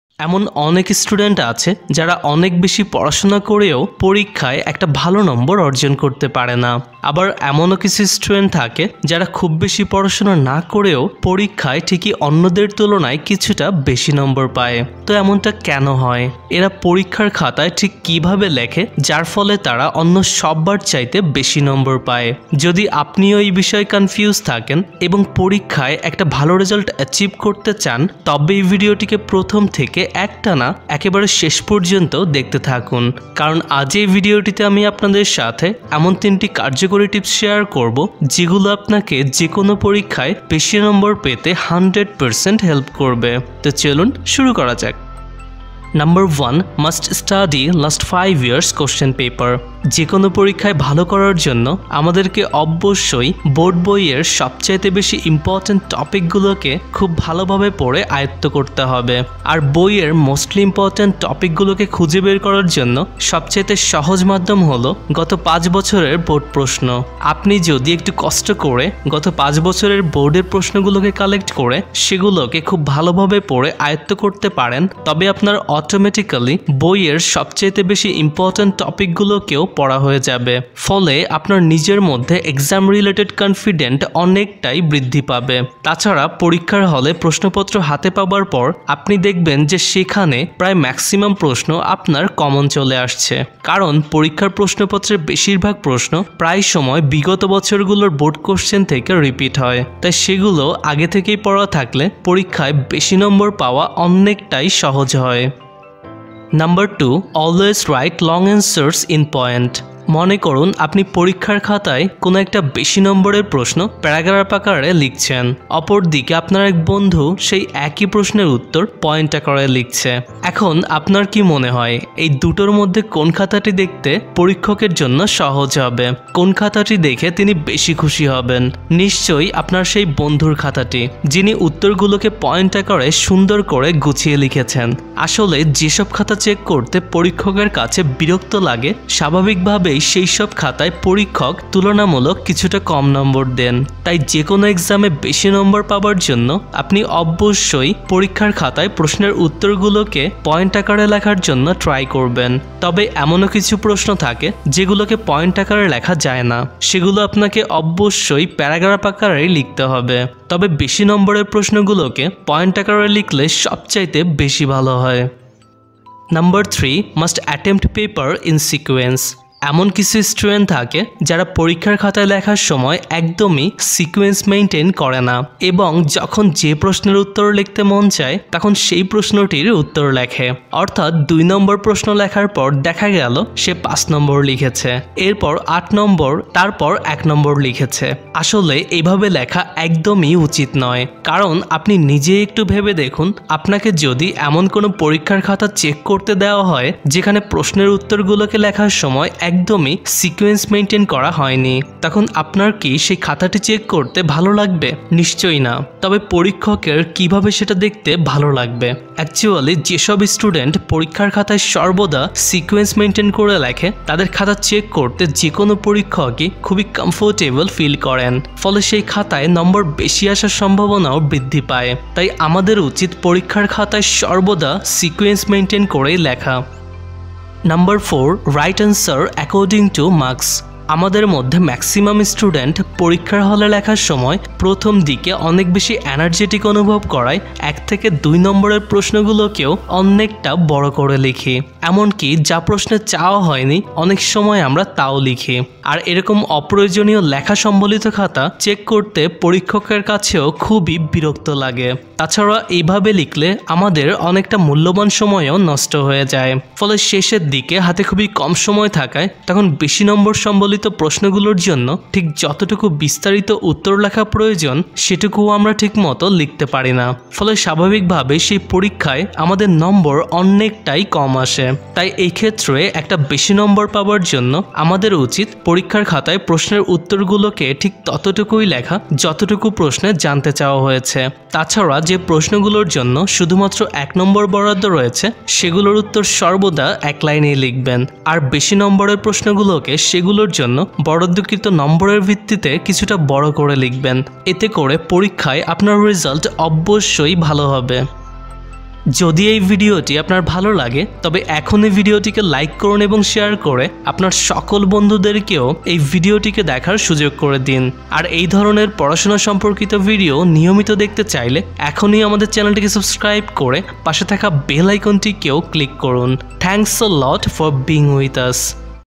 পরীক্ষায় ৯৯% নম্বর পাবার উপায় | How to get good marks in Exam | Study tips in bangla
Hello Everyone. In this video i have shared 4 secret tips to get good marks in Exam.

Such as:-
1) Must study last 5 years question paper
2) Always Write long answers in point
3) Must Attempt paper in Sequence
4) Write answer according to Marks

I hope this video will help you a lot to make an excellent result in your next Exam.